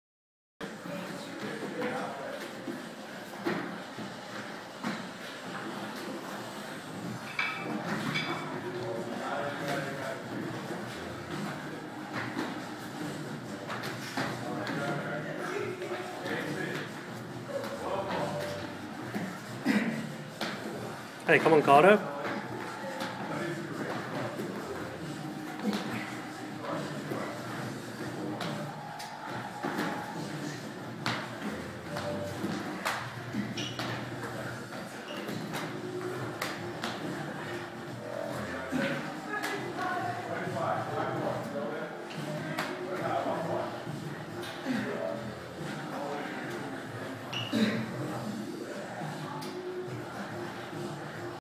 Chikara Athlete Kaoru
For time :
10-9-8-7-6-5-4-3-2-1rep
Burpee Toes to bar

Result : 6:13